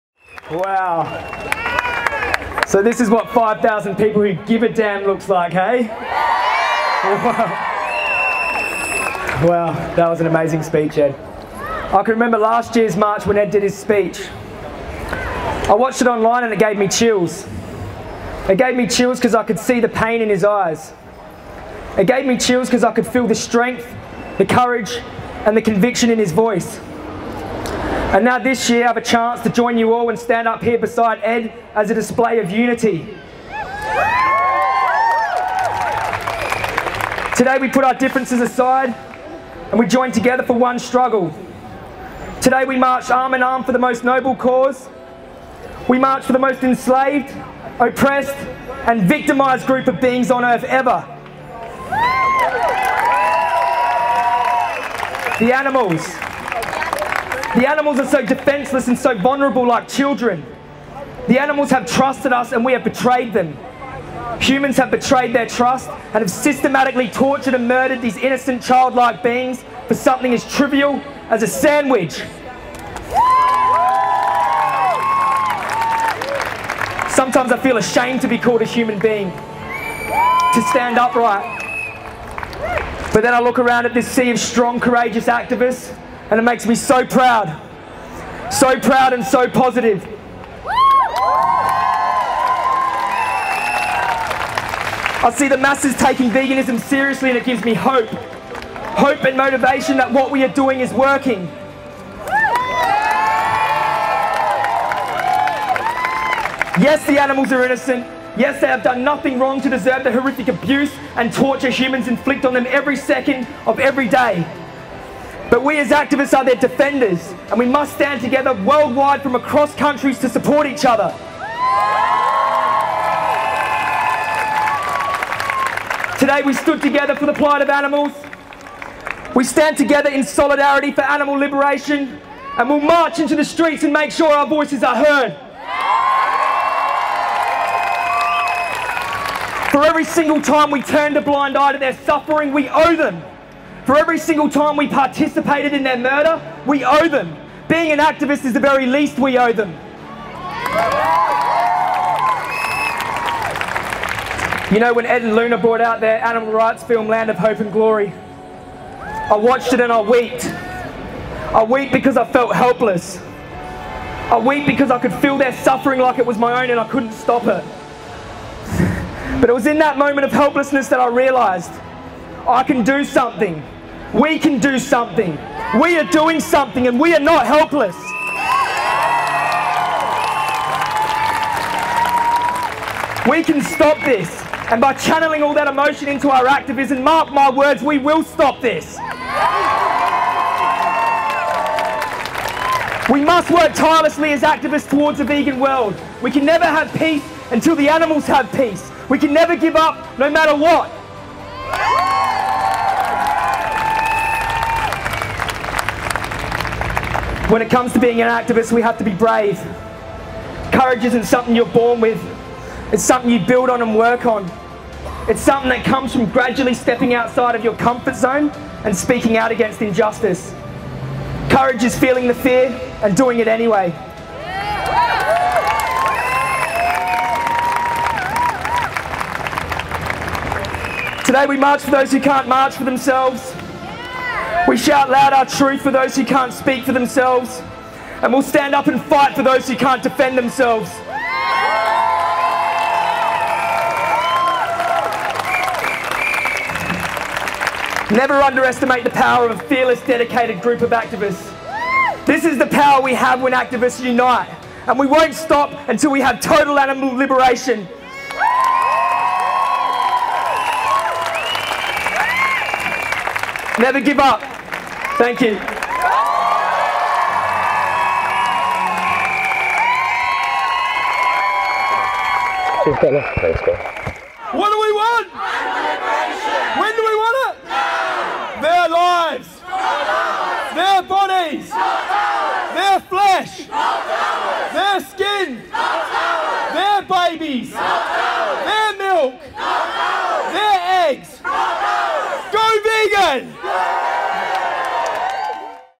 NEVER GIVE UP... [LONDON ANIMAL RIGHTS MARCH SPEECH 2017]
Full credit to Ed and Luna Of Surge Activism for organising such an epic day. Was an honour to march with you all.

WATCH THESE:
The Best Speech You'll ever hear-